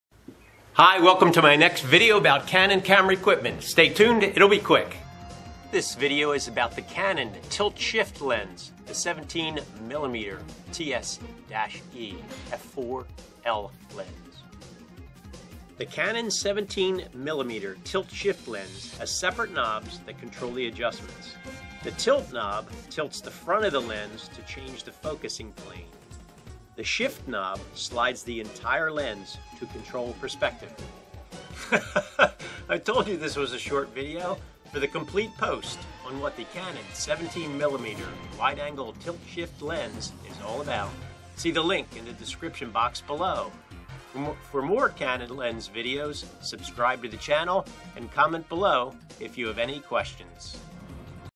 How the Canon 17mm tilt shift lens works
How do you use the Canon  super wide angle17mm TS-E tilt-shift lens. This super short video shows you how to adjust the tilt and the shift movements of this specialty lens.

Here's the complete article on the Canon 17mm TS-E f/4.0 L tilt-shift lens